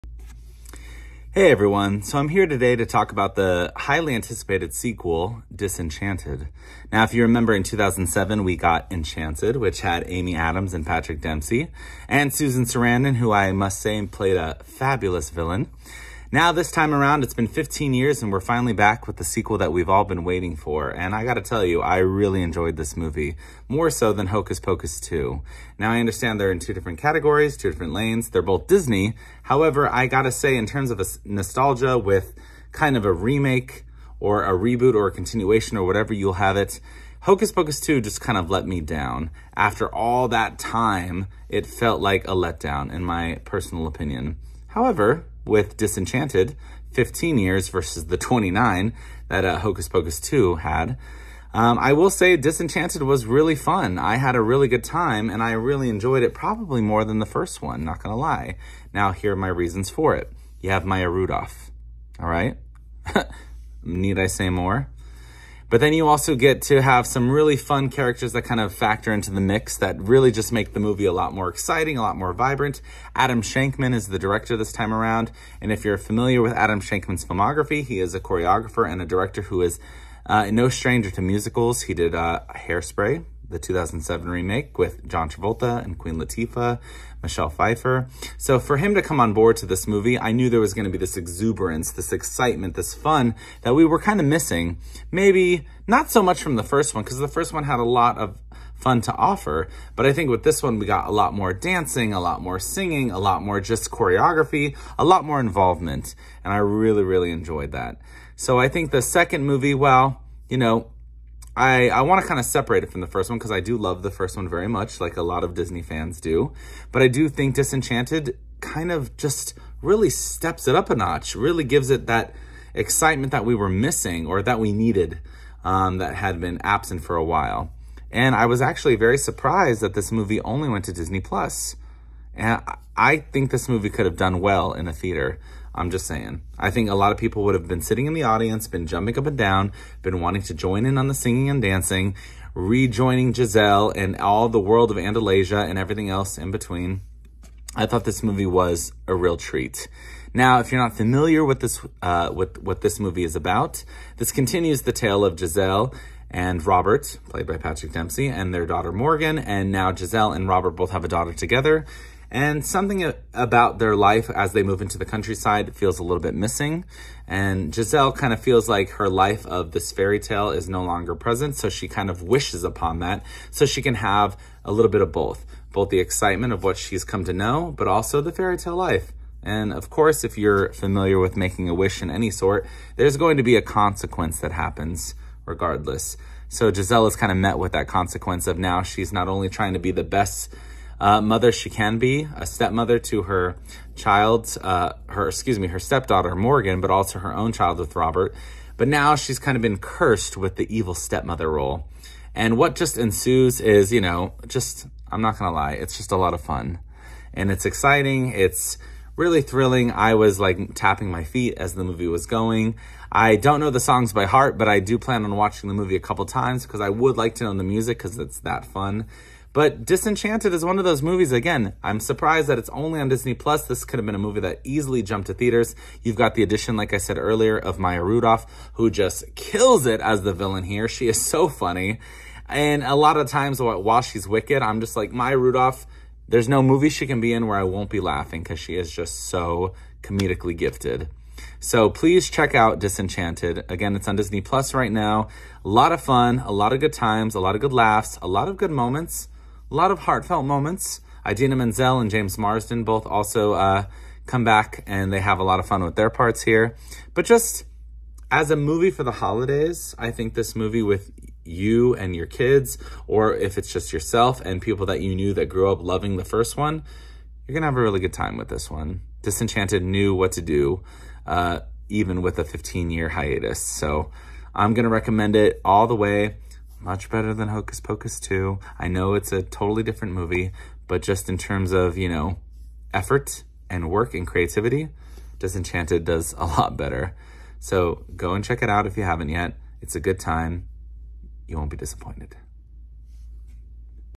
Disenchanted (2022) Movie Review 🍿
Film review for ‘Disenchanted.’ Starring Amy Adams, Maya Rudolph, Idina Menzel, Yvette Nicole Brown, Jayma Mays, and Patrick Dempsey. Directed by Adam Shankman. The highly anticipated sequel to 2007’s ‘Enchanted.’